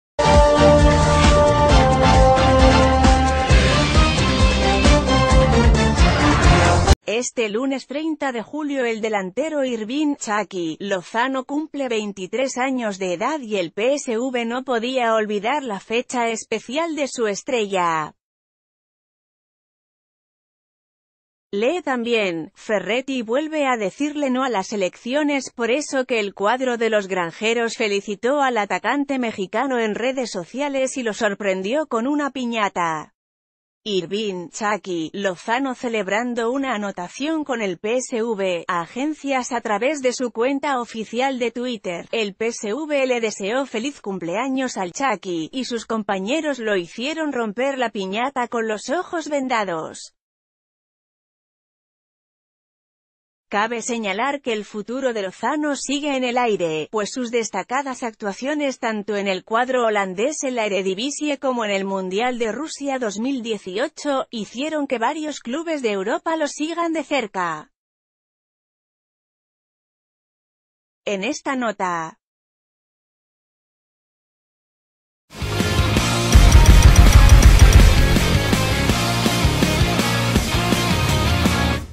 Con piñata, el PSV celebró el cumpleaños del Chucky Lozano
Este lunes 30 de julio el delantero Hirving "Chucky" Lozano cumple 23 años de edad y el PSV no podía olvidar la fecha especial de su estrella.